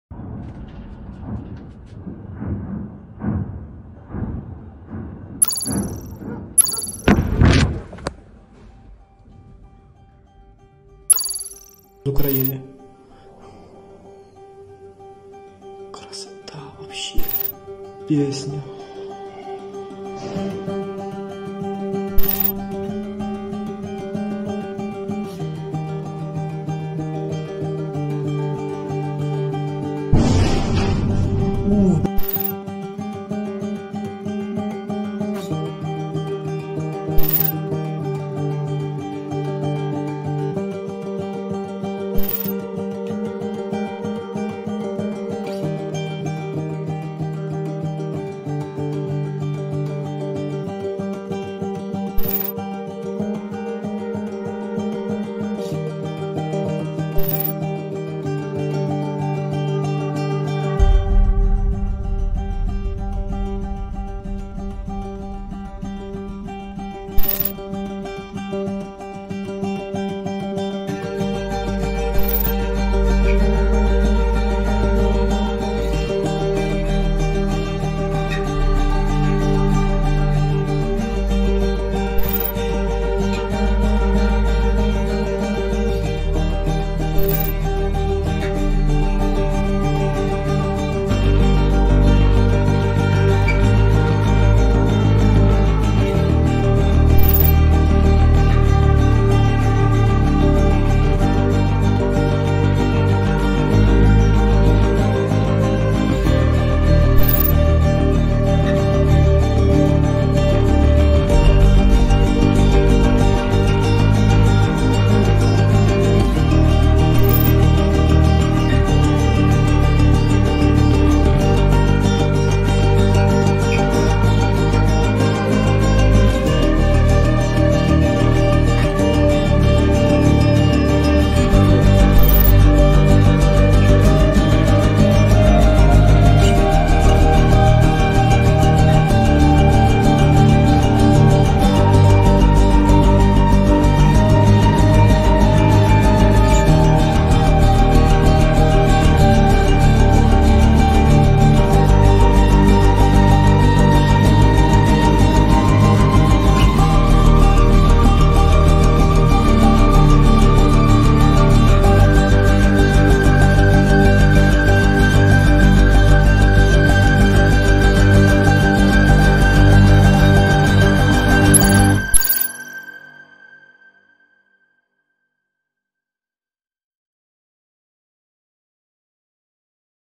Koniec ruskej ofenzívy? Na fronte nastala situácia, aká už dávno nebola | Aktuality
📣  Putinova armáda devastovala ukrajinské územie masívnou delostreľbou.
Rusom sa zhruba po troch mesiacoch od vyvolania vojny na Ukrajine podarilo na Donbase sústrediť takú palebnú silu, že denne Severodoneck zasypávali z diel aj 50-tisíc strelami. Koľko mužov dobýjalo ukrajinské mesto, ako mohlo vyzerať také brutálne bombardovanie a čo nasledovalo po ňom, sa dozviete vo videu.

# vojna na ukrajine